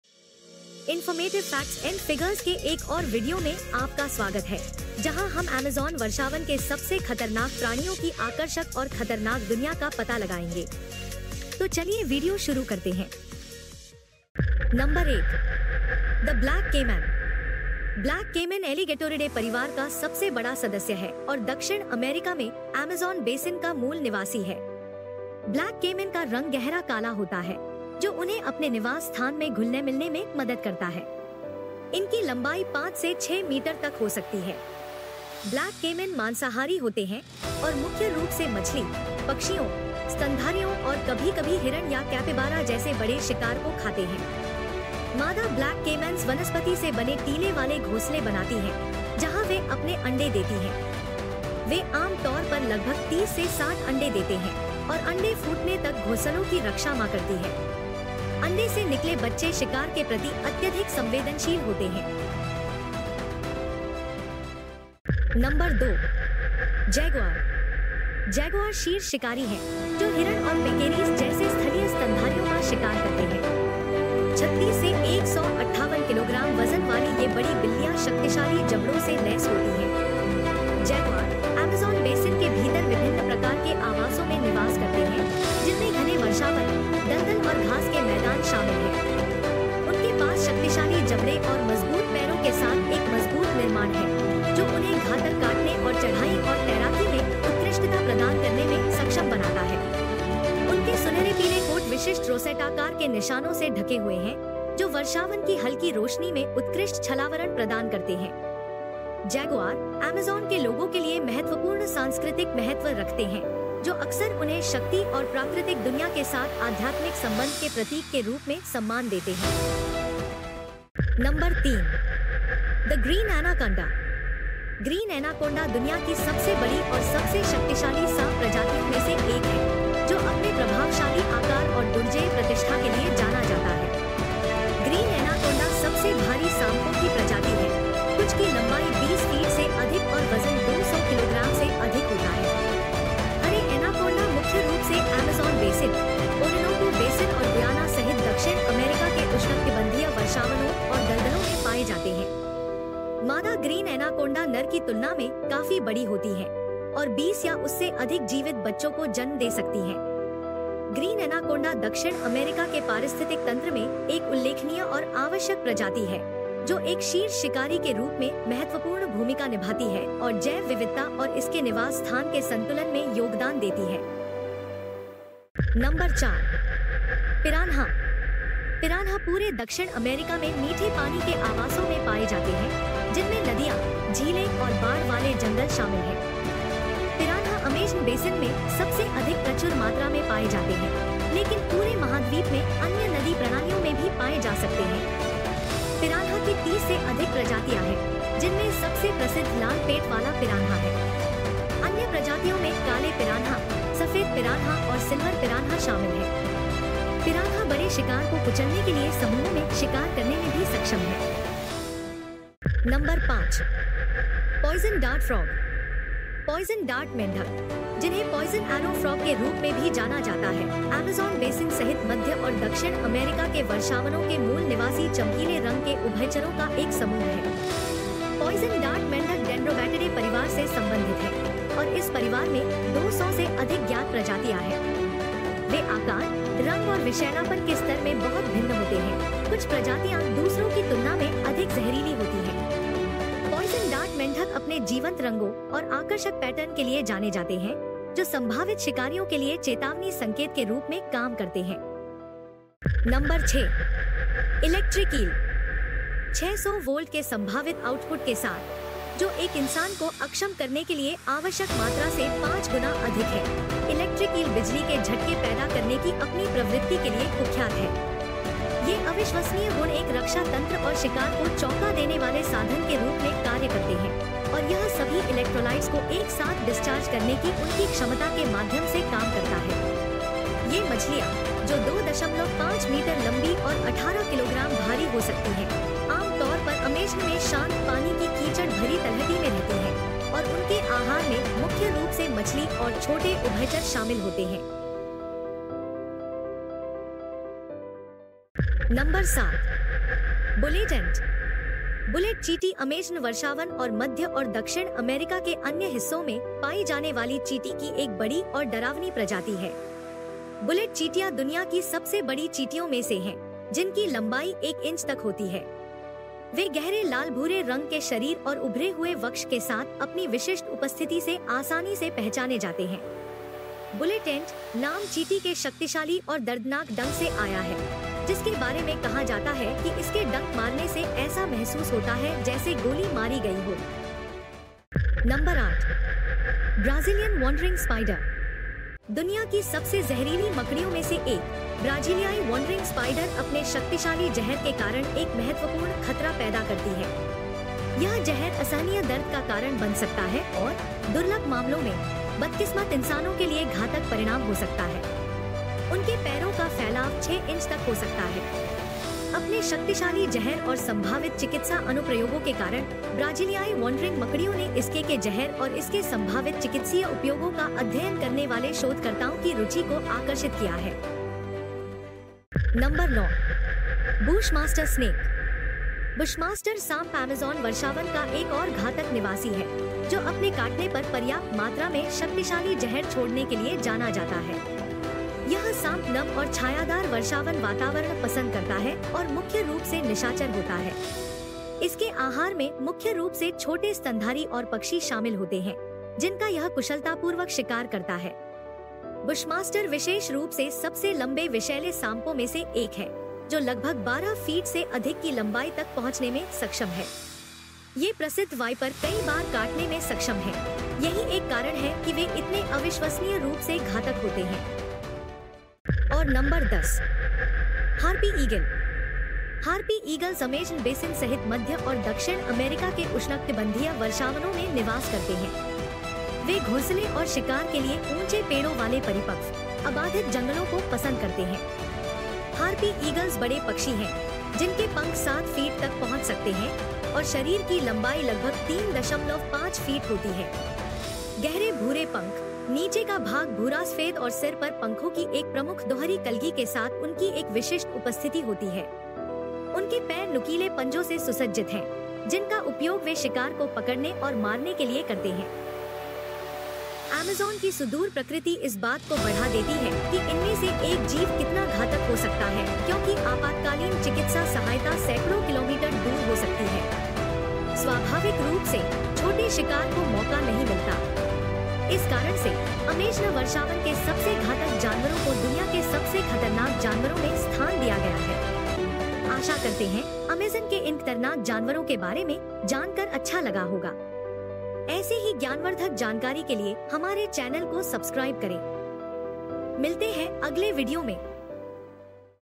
10 MOST DANGEROUS ANIMALS OF AMAZON | अमेज़न जंगल के 10 सबसे खतरनाक जानवर
इस रोमांचकारी वीडियो में, हम ग्रह पर सबसे अधिक जैव विविधता वाले पारिस्थितिक तंत्रों में से एक की रहस्यमय गहराइयों में उतरकर इसके शीर्ष 10 सबसे खतरनाक निवासियों को उजागर करते हैं। विषैले सांपों से लेकर छिपकर शिकारियों तक, अमेज़ॅन असंख्य प्राणियों का घर है जो सम्मान और भय दोनों का कारण बनते हैं। चाहे आप प्रकृति के शौकीन हों या जंगली चमत्कारों के बारे में उत्सुक हों, यह वीडियो निश्चित रूप से मंत्रमुग्ध करने वाला और शिक्षित करने वाला है। तो अपनी दूरबीन पकड़ें, अपना साहस बांधें, और अमेज़ॅन वर्षावन के अदम्य जंगल में इस रोमांचक अभियान में हमारे साथ शामिल हों!